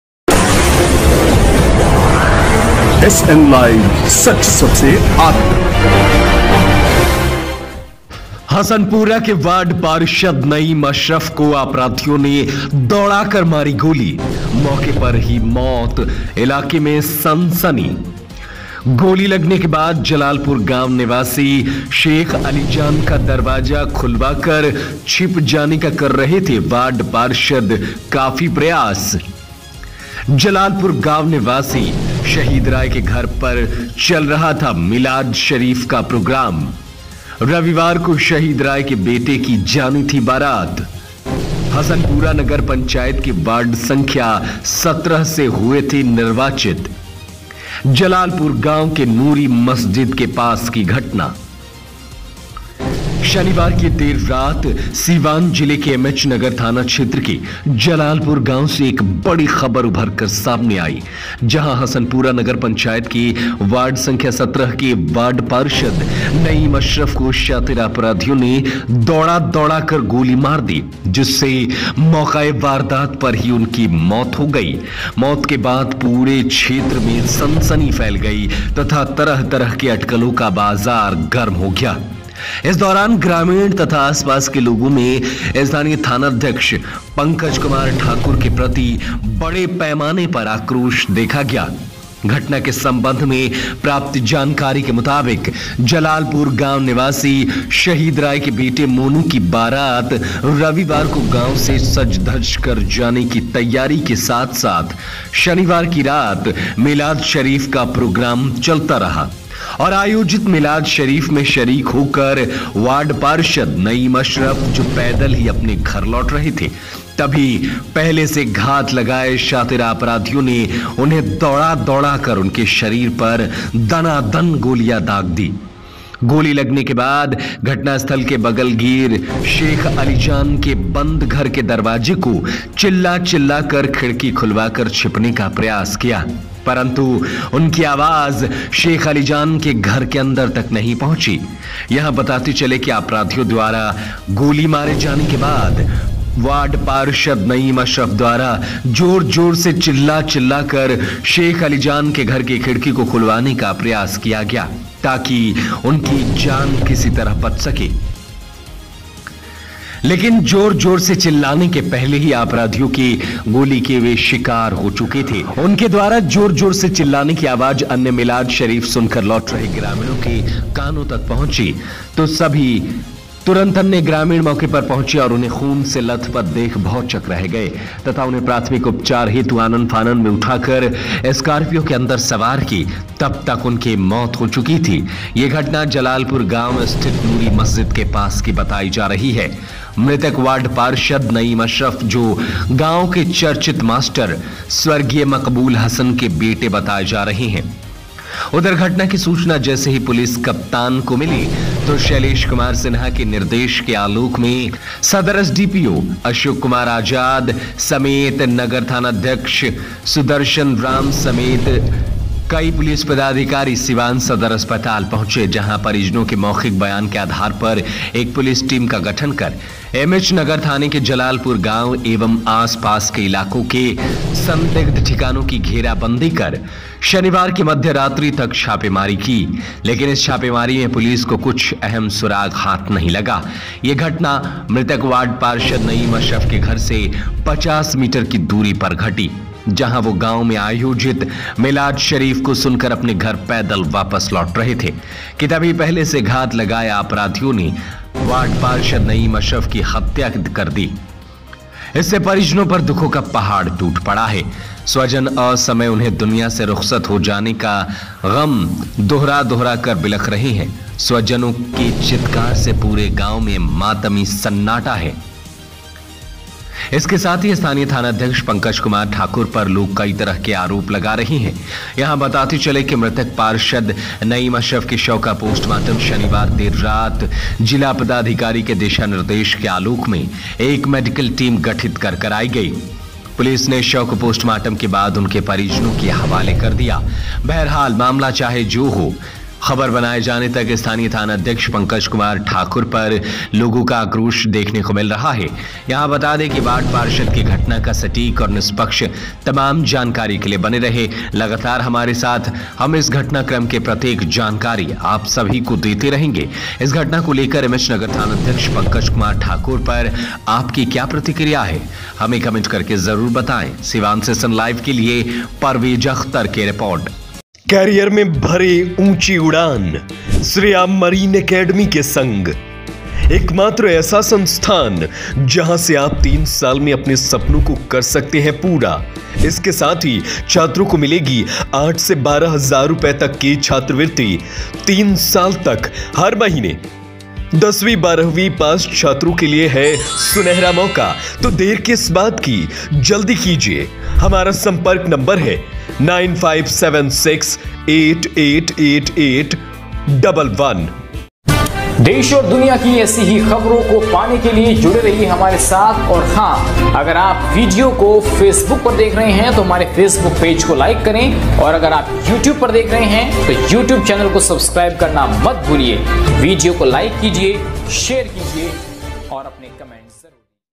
सिवान के हसनपुरा में वार्ड पार्षद नईम अशरफ को अप"राधियों ने दौ'ड़ा कर मा"री गो'ली छापेमारी जारी
सिवान के हसनपुरा में वार्ड पार्षद नईम अशरफ को अप"राधियों ने दौ"ड़ा कर मा'री गो"ली छापेमारी जारी LIVE

                  - FOLLOW ME -